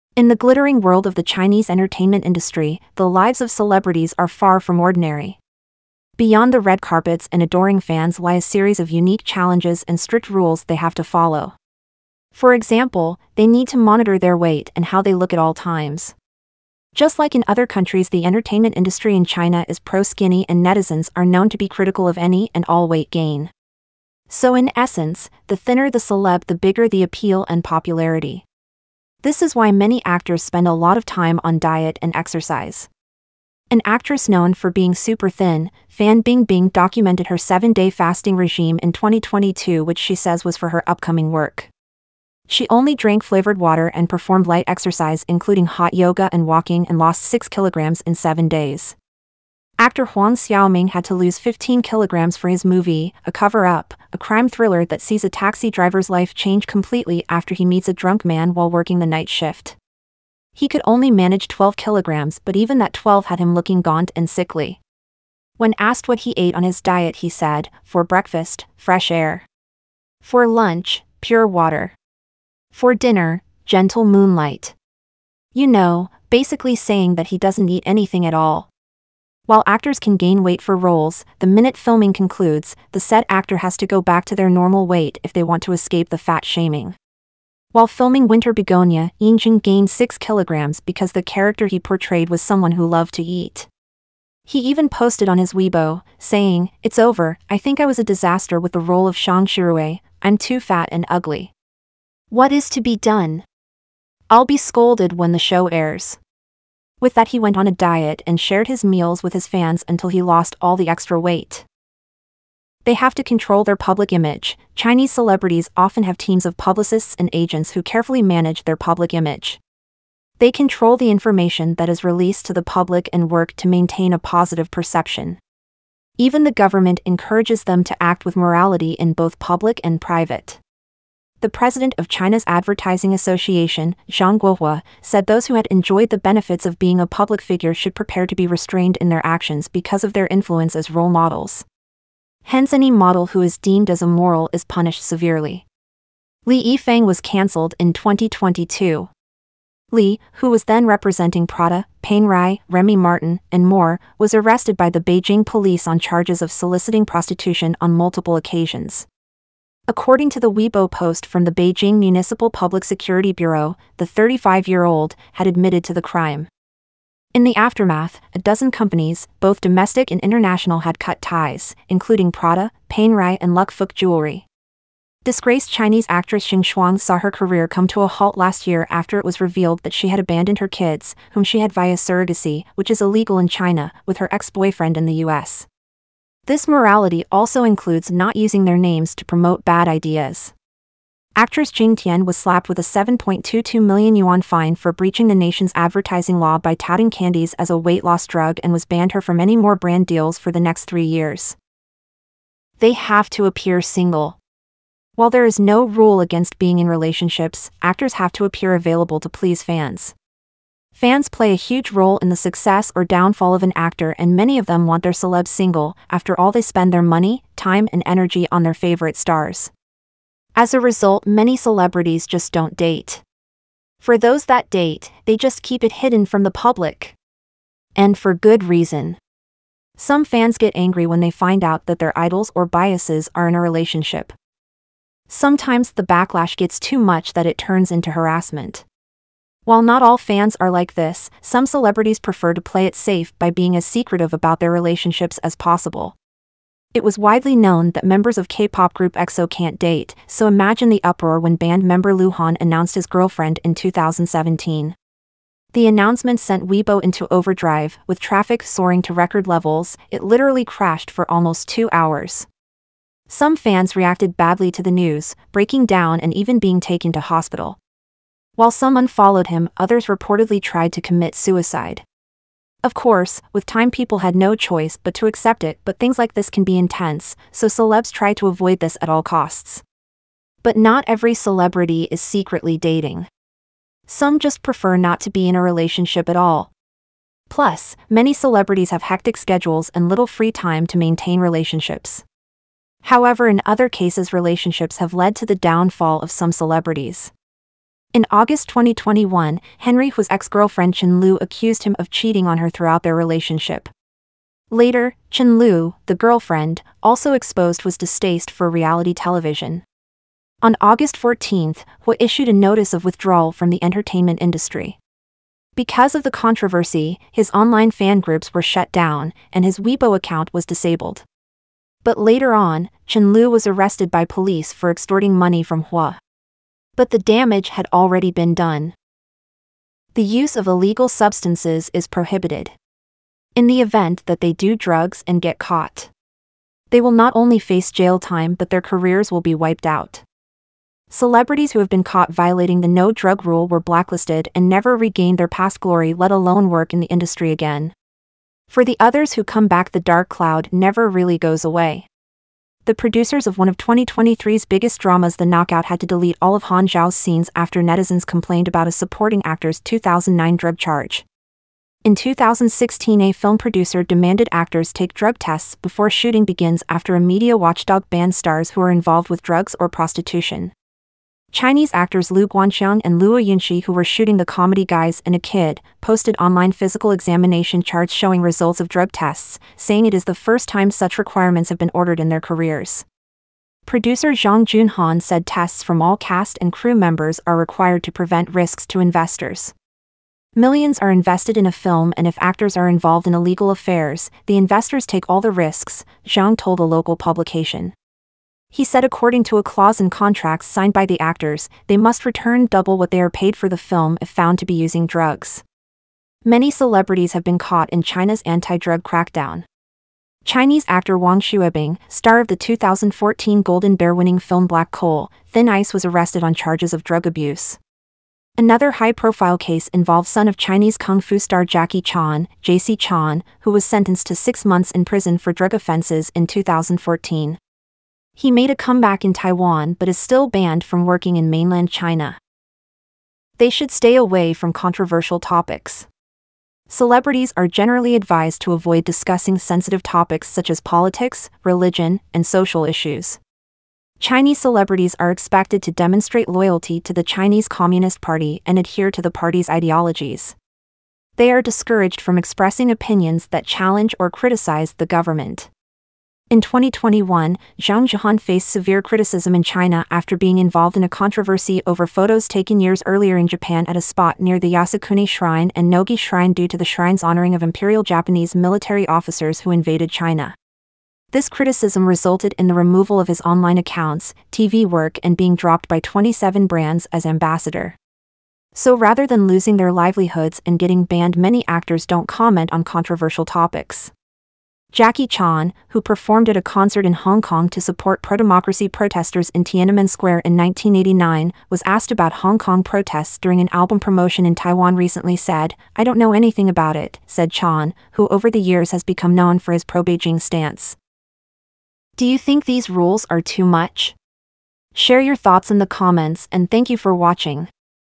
Top 5 Strict Rules Chinese Actors MUST Follow
With fame comes challenges. in today's episode we aew going to be looking at all the strict rules chineseactresses and chineseactors have to folllow.